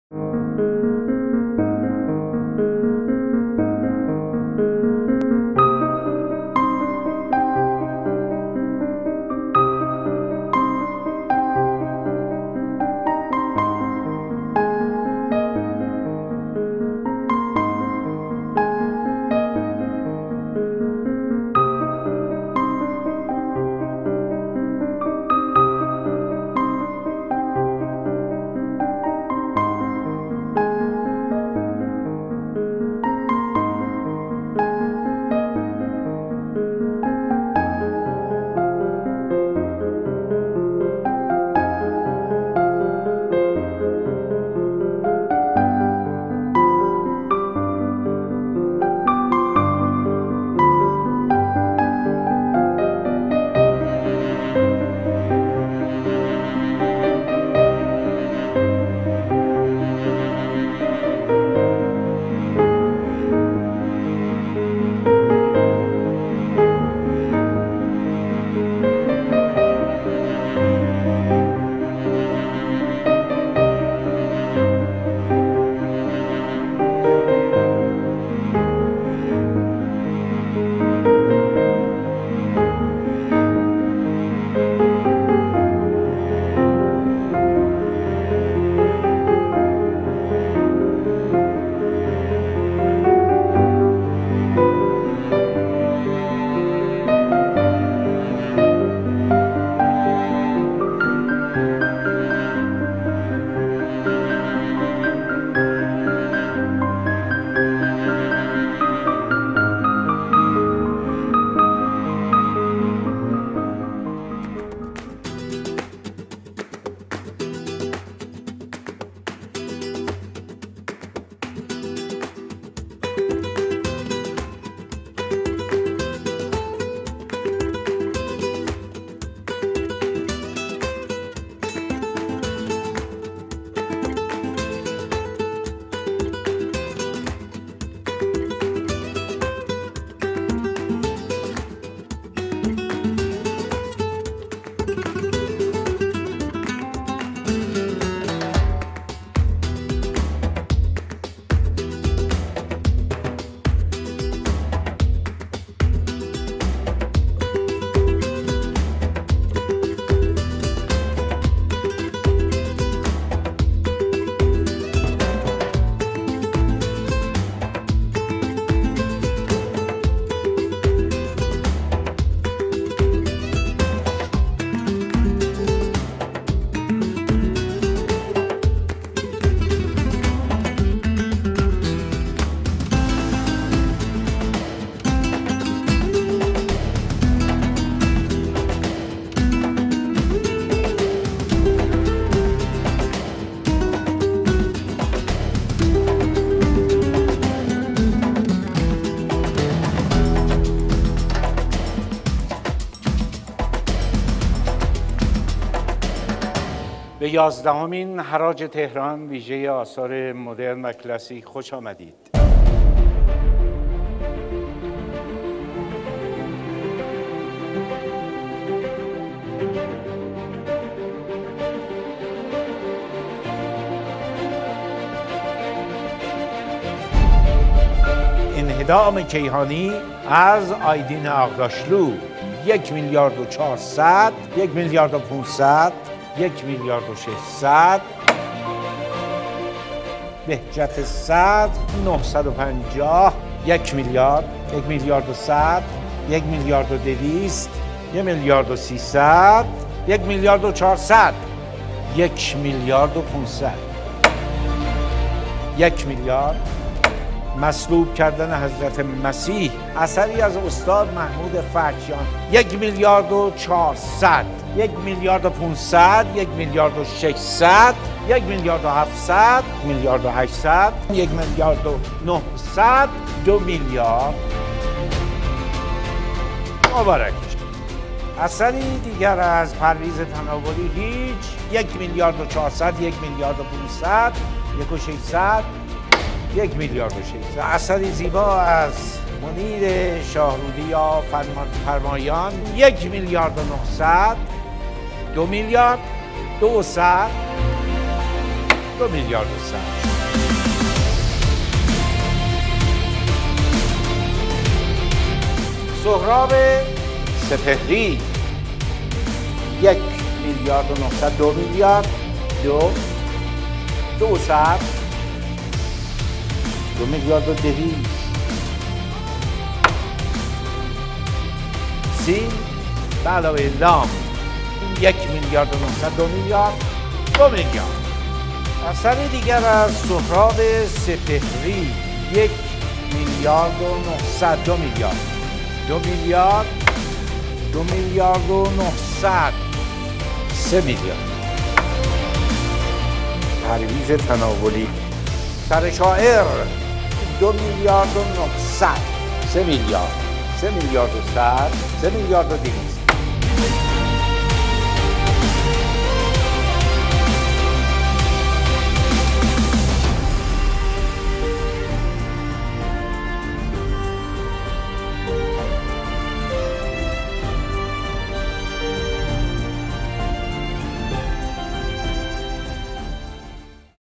Tehran Auction 11th Clip
Tehran Auction held its 11th auction, offering Classic and Modern Iranian Art at the Azadi
Hotel, Tehran, Iran.
Over 800 enthusiastic art lovers and collectors of Iranian art were present and drove the
total sale up by almost 35% over the last Tehran Auction Classic and Modern sale in June
2018. There were 140 registered bidders in the room. Sale of 79 works of art by 60
artists, consisting of 74 paintings and 5 sculptures, reached a total of 3,836,272 USD.